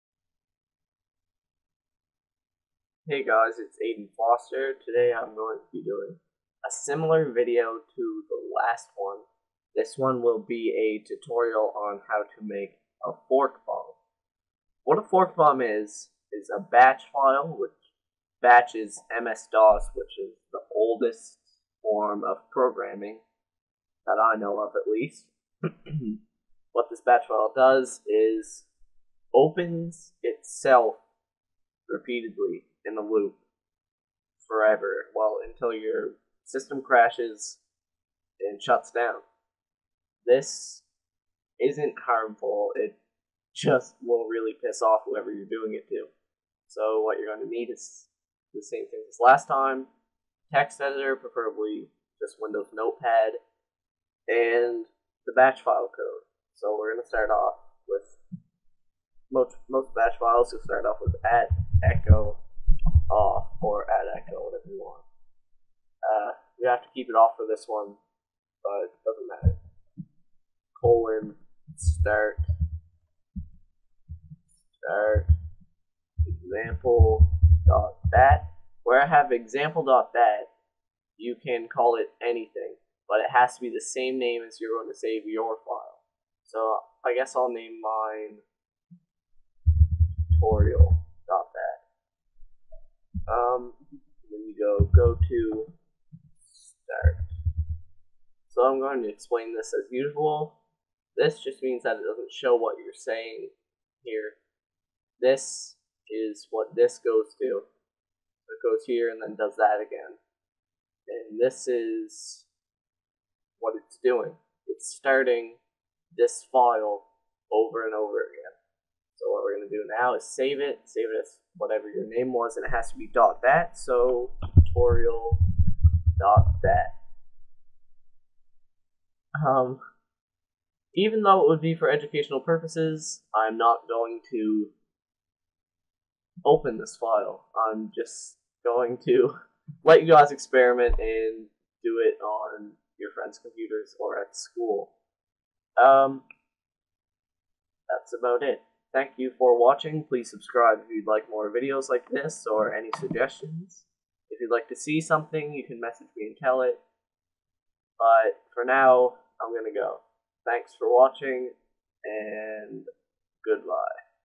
Fork Bomb Tutorial
Hey Viewers!
In this video I will be showing you how to make a Fork Bomb. A fork bomb is a MS-DOS command that continuously opens itself, until the computer runs out of processing power, shuts down. Use this simple batch file on your friends, and enjoy the raging that results!

This file causes no harm to the computer, since the computer knows when it is about to harm itself.

I'll be making short batch/vbs tutorials every day for the next few days.
Aiden Foster